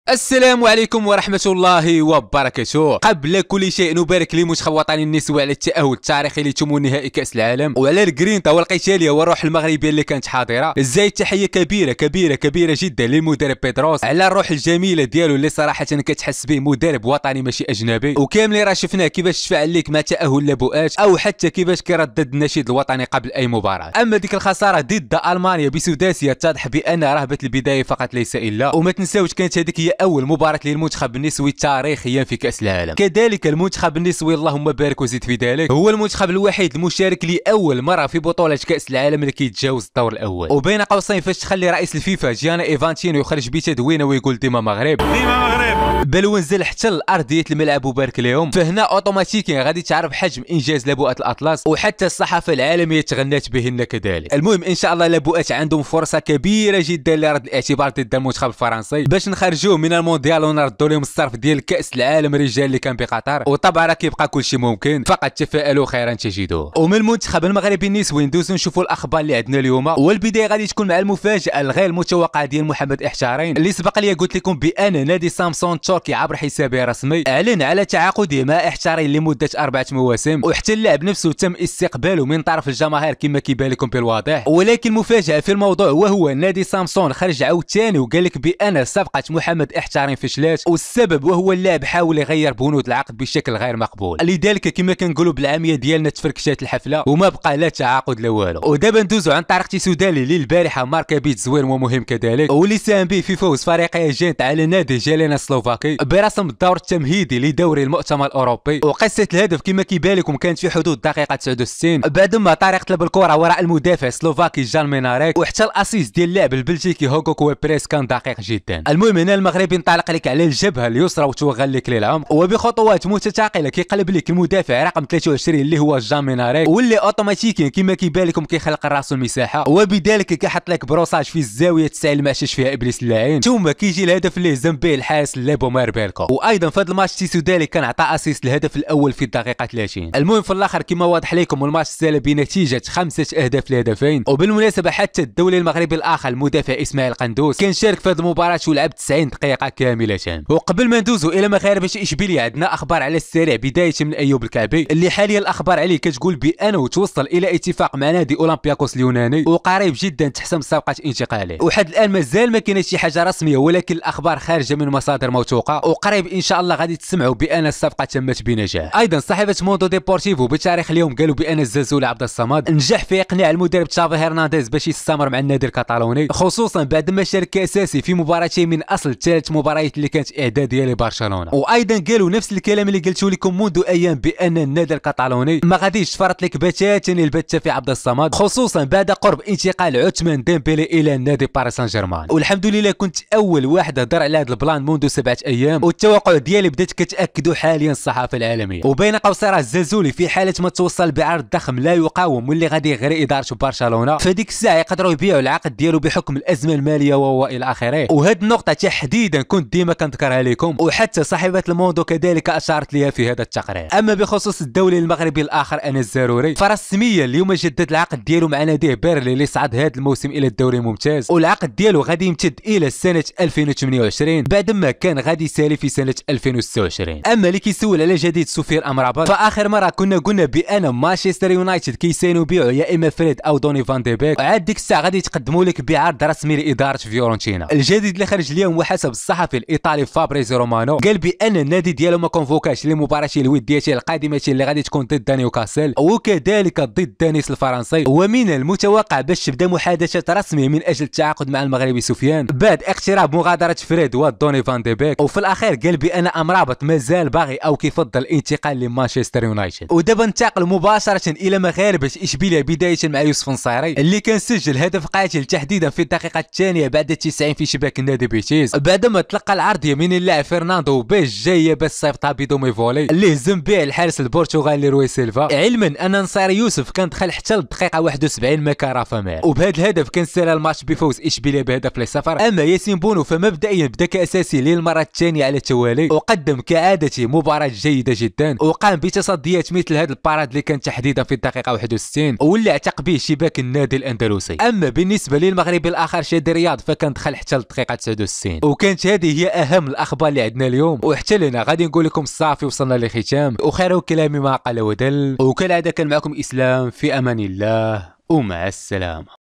سمعو الجديد العاجل⬅️مع عالمية يوسف النصيري بالهدف مع طارق تيسودالي+مُفاجأَة سفيان امرابط انس الزروري
الانستقرام 👇👇
ISLAM SPORT

سمعو الجديد العاجل⬅️مع عالمية يوسف النصيري بالهدف مع طارق تيسودالي+مُفاجأَة سفيان امرابط اليوم وانس الزروري
ISLAM SPORT